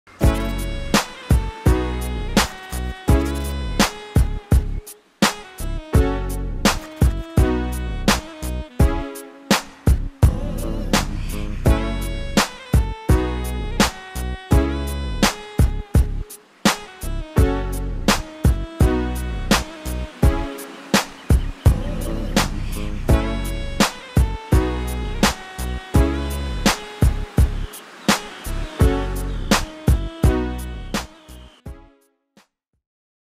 San Francisco Bay Area Sportfishing Charter Boat Drone Shot: Oakland Anglers
Cool sneak peak of The Oakland Anglers Charter Boat cruising the SF Bay via drone shot and cut by our good friends from @DepthSquad and James.